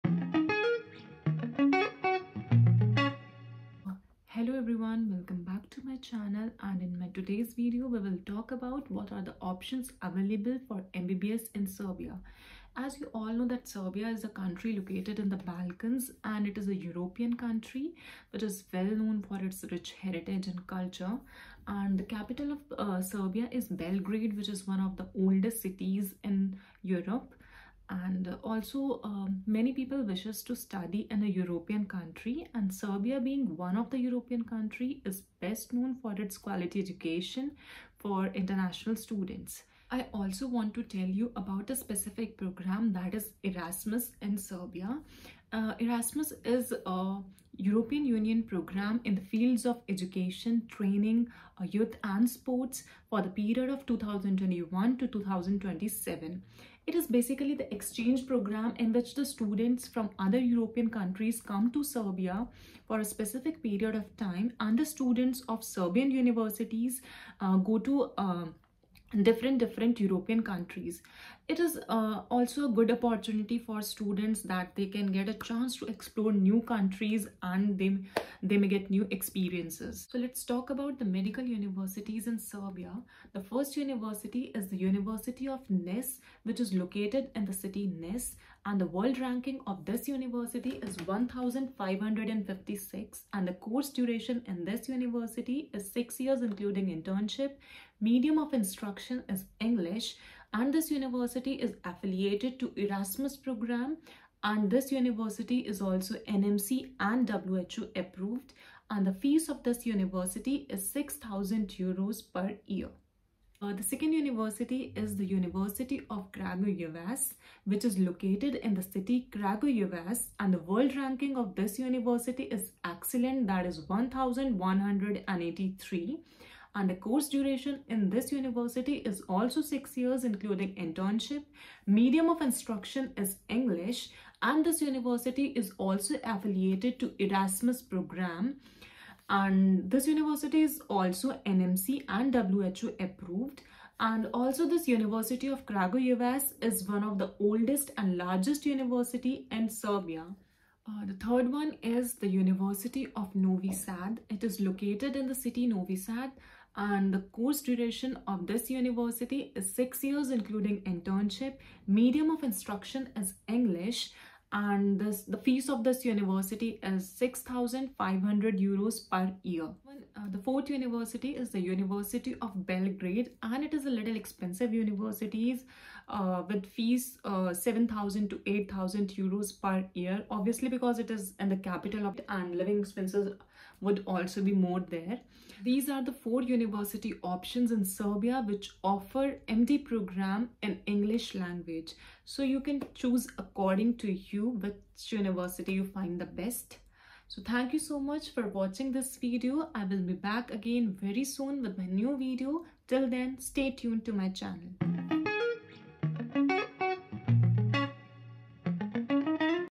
MBBS option in SERBIA 🇷🇸
Hello everyone!! In this video I have told you all the 4 universities of Serbia.